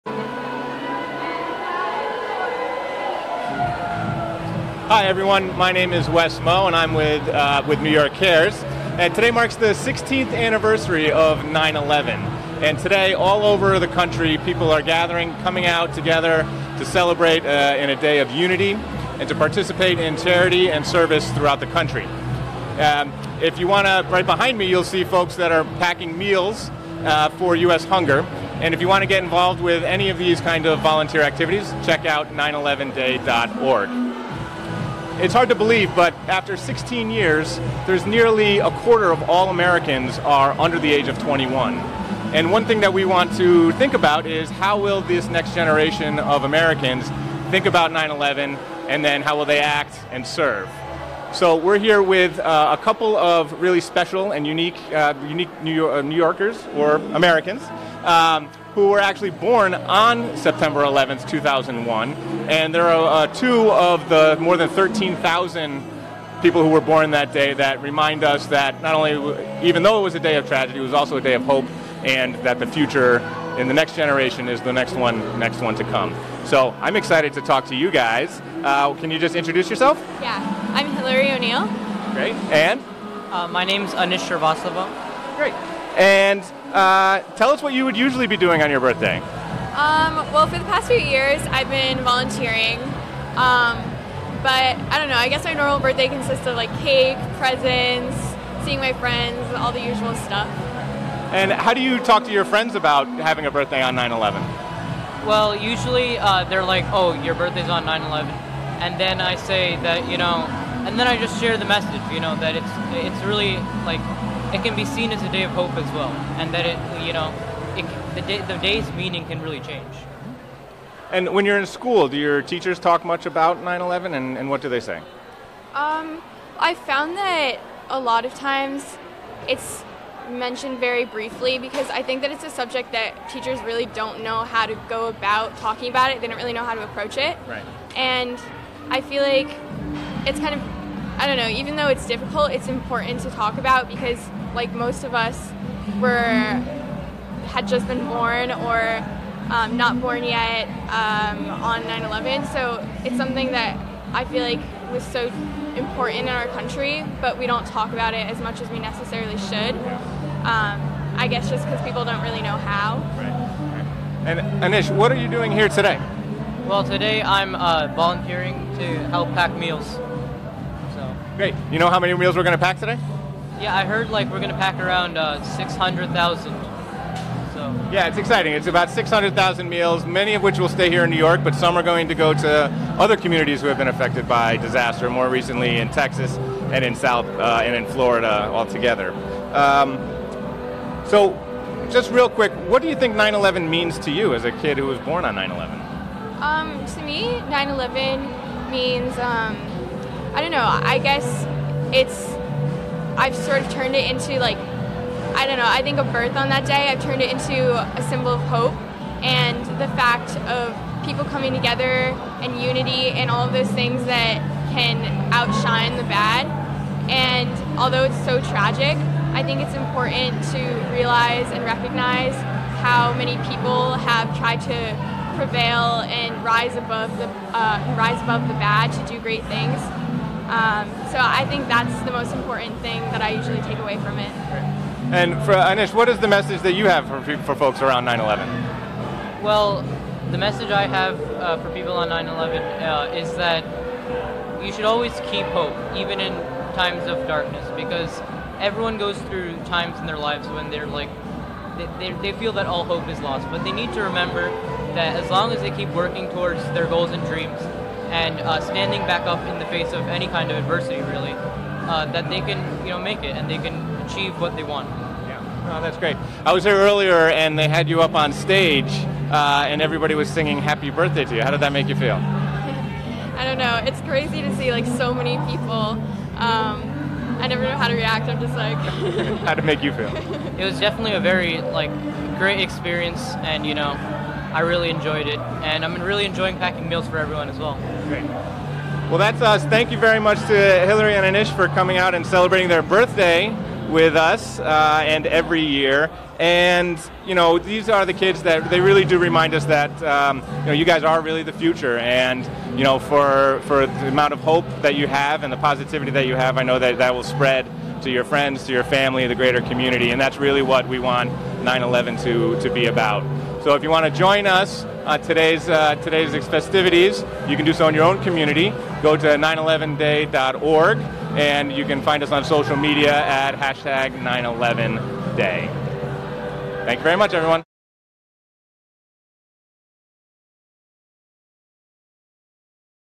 LIVE on 9/11 Day with New York Cares and Children Born on 9/11
On a day forever scarred by tragedy, New York Cares discusses what it's like being born on 9/11.